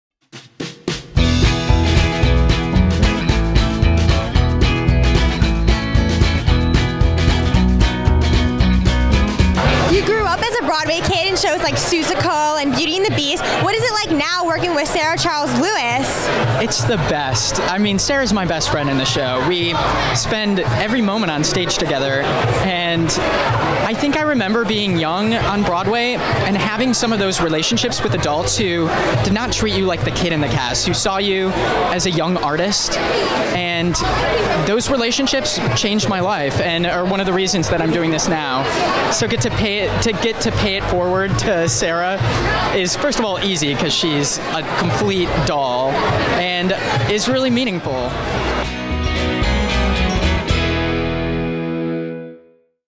Paying It Forward with Andrew Keenan-Bolger
Andrew Keenan-Bolger began performing when he was a kid, so he knows exactly what his Tuck Everlasting cast-mate Sarah Charles Lewis is going through as a kid on Broadway!

For more family-friendly Broadway content check out BwayZone.com